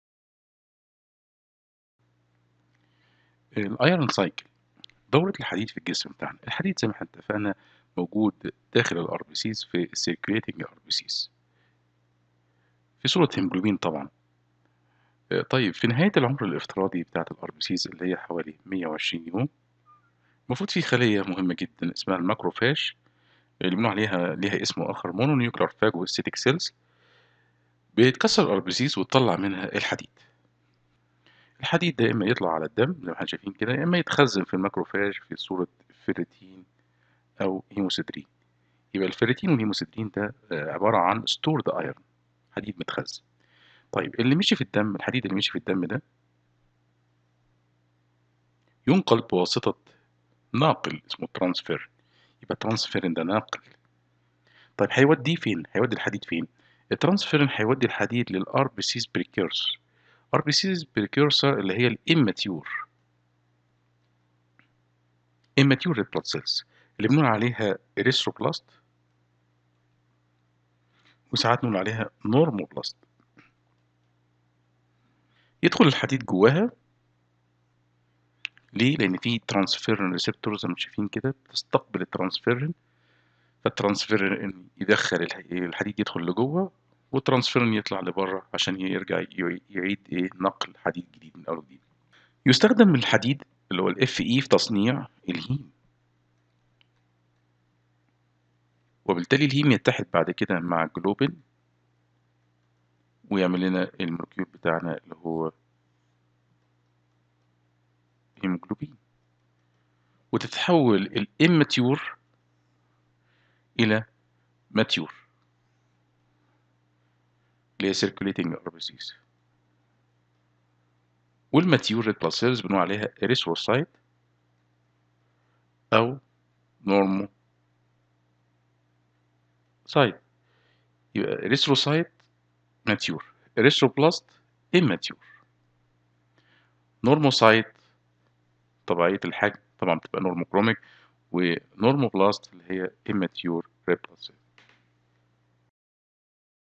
Hematology - 1 | U7-L3 | Iron cycle
Unit - 7 : Introduction to microcytic hypochromic anaemias
Lecture - 3 : Iron cycle
1- Iron forms
2- Transferrin